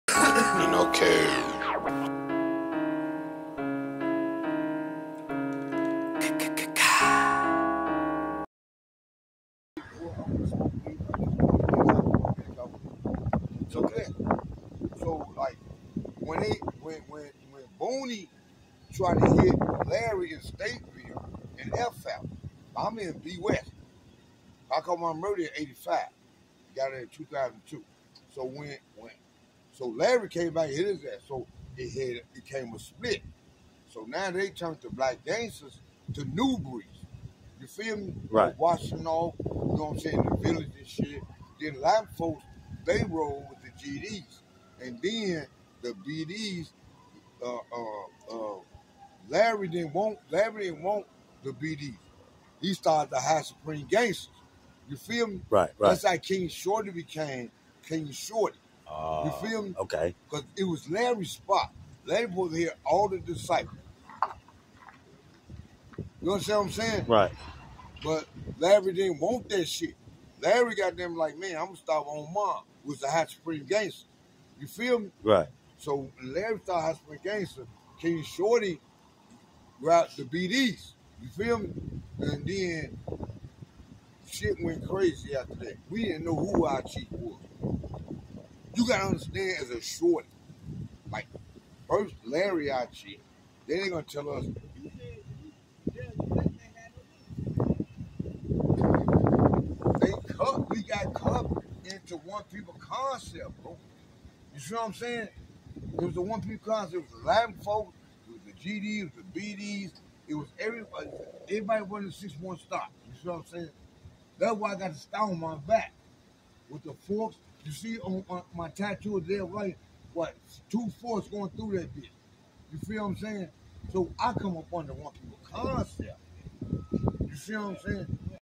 OG gives history of chicago GDS & BDS
PART 1. OF OG giving history of larry hoover ,gds bds and prison story